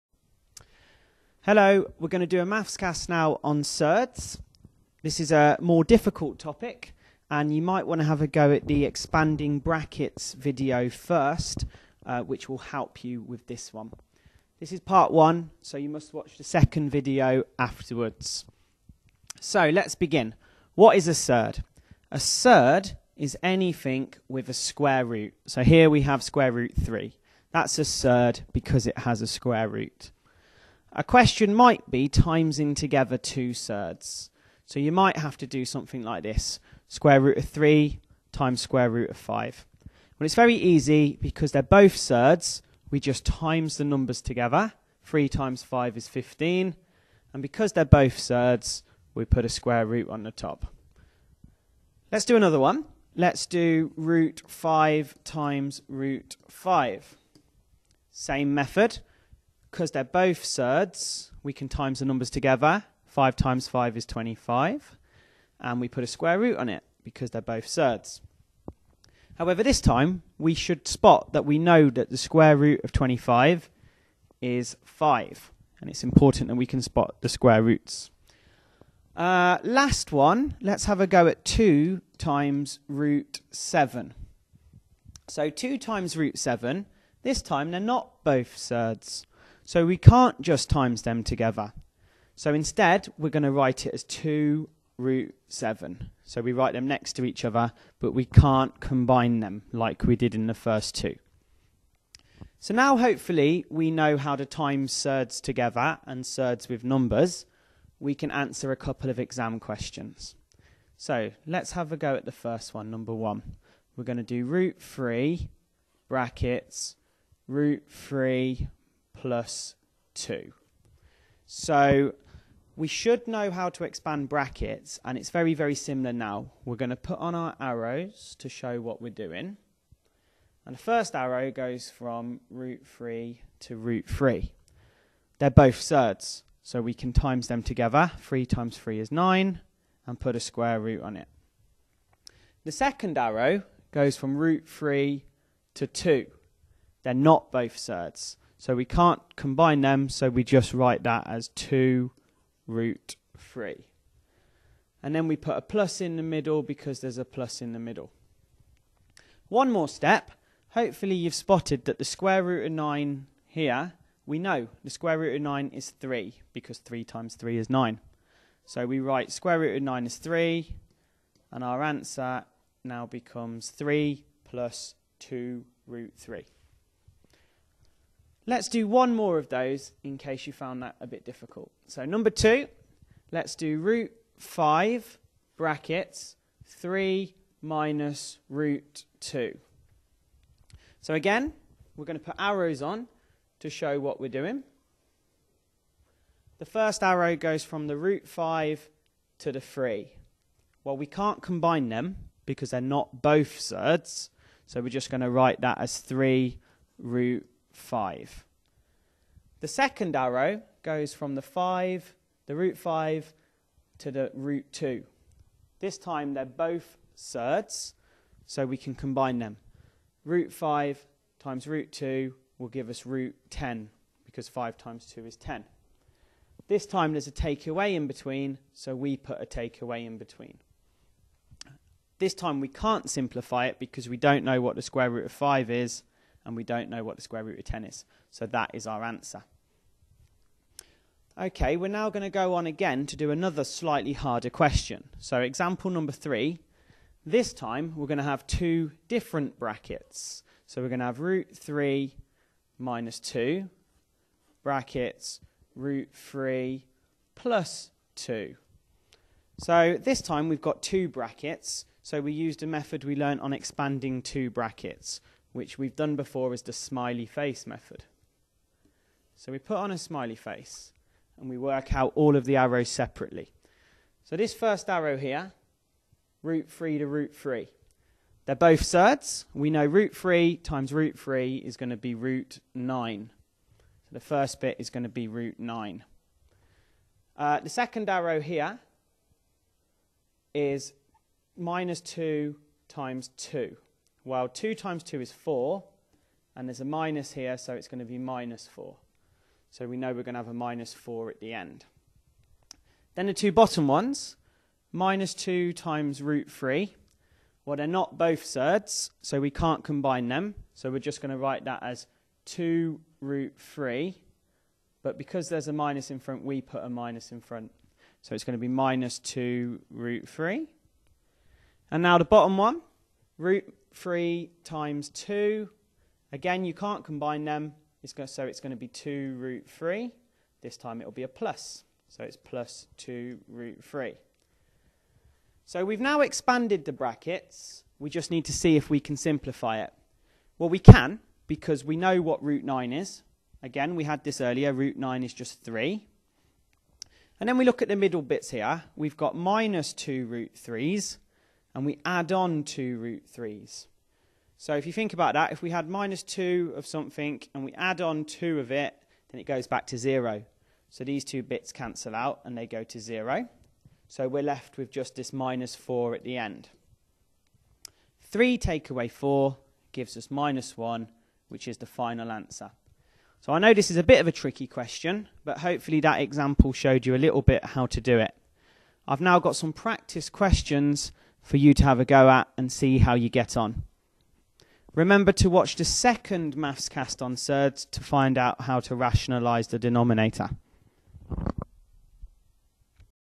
Surds (Part 1) Mathscast
A mini tuition video on surds showing how to expand brackets and simplify surds and ending with some practice questions.  See part 2 for rationalising.